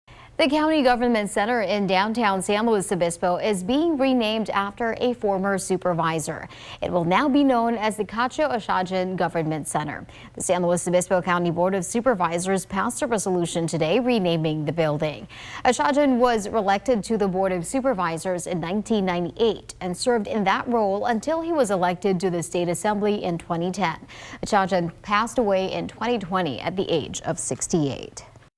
SLO County government building to be renamed in former supervisor’s honor
The County Government Center in downtown San Luis Obispo will now be known as the Katcho Achadjian Government Center.